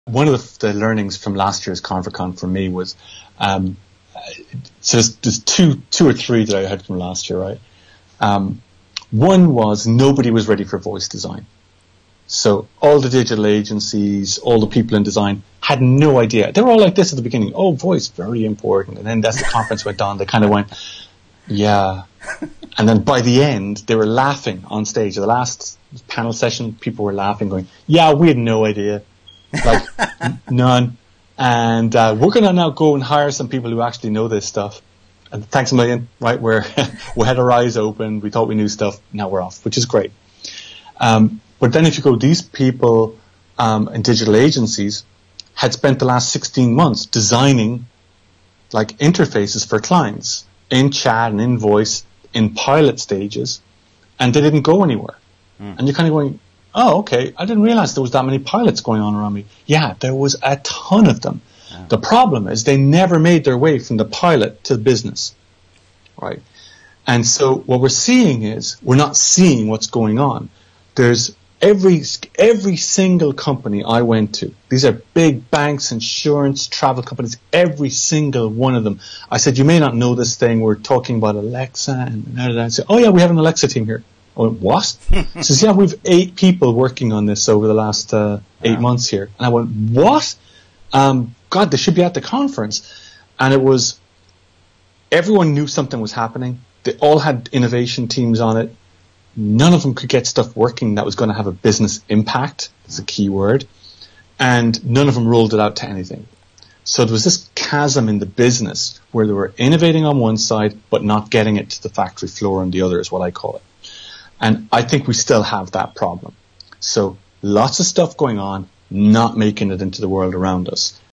How the lack of voice design impacts voice integration projects | ZDNet
The cofounder of conversational middleware platform Webio, Paul Sweeney, discusses how the lack of voice design and its impact on voice integration projects was revealed at Convercon, the UK-based conversational interfaces conference he founded last year.